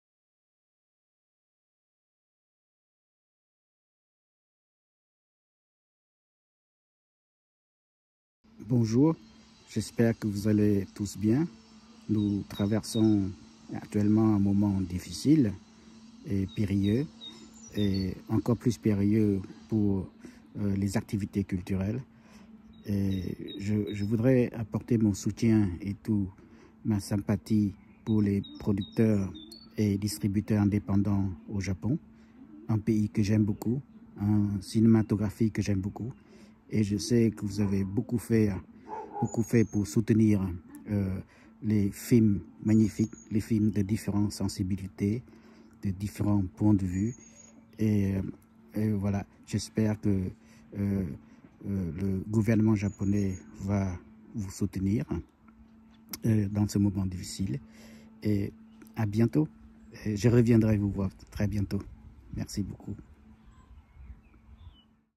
リティ・パン＠世界の映画人からの応援動画メッセージ
Help! The 映画配給会社プロジェクト
世界の映画人からの応援動画メッセージ
リティ・パン『消えた画　クメール・ルージュの真実』監督／カンボジア